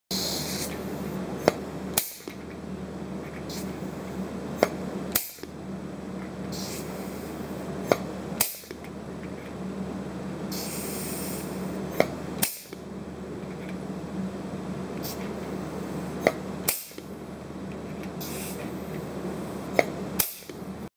State Enterprises - How to Install Solid Rivets
Learn How to Install Solid Rivets with the Use of Solid Rivet Tool.